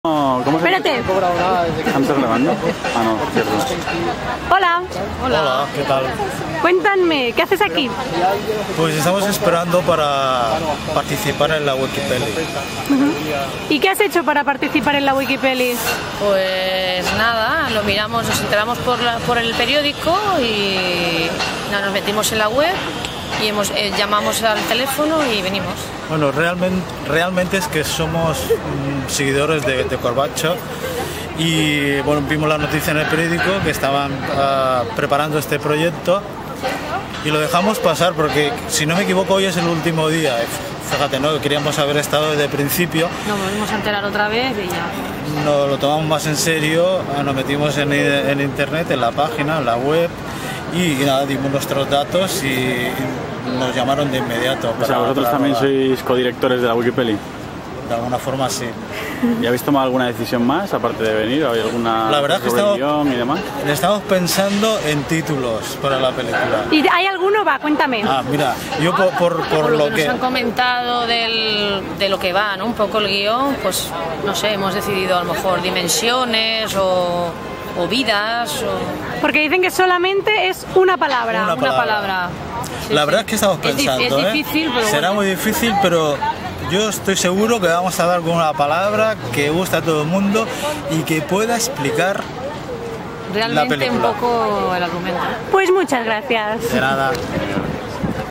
Entrevista Figuración de la Wikipeli 1
Hablamos con la gente que ha participado en el blog de la wikipeli de mahou y ha conseguido ser figurante en la película.